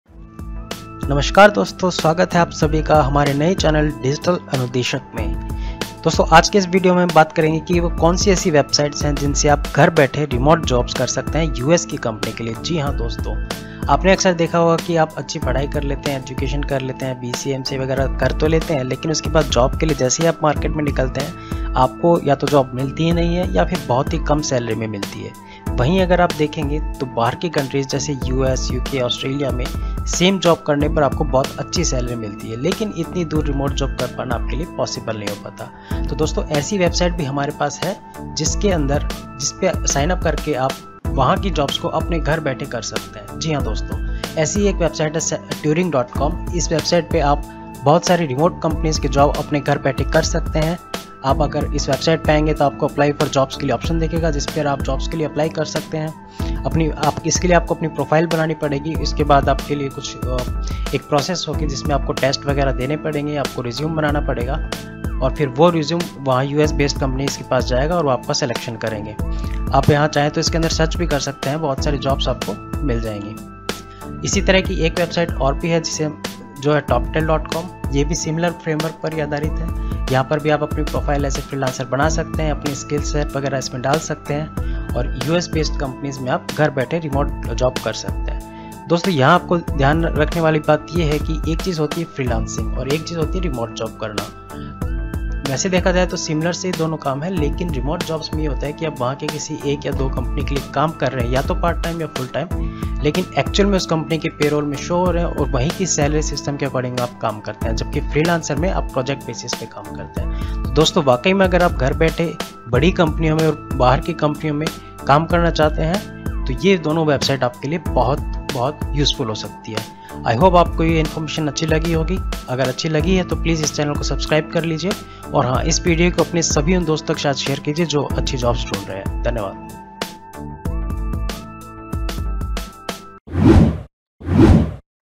How to get Remote Jobs after BCA in 2023 | Work from Home after BCA or MCA
Are you looking for remote jobs after doing BCA? In this video I am explaining how you can get a good job in US while working from Home. Yes, You can get a remote job for US based companies and can earn in USD.

This video is about:

How to get Remote Job after BCA
How to get Remote Job after MCA
Work from home after BCA / MCA
Freelancer job for BCA MCA candidates

Please comment your queries and feedback.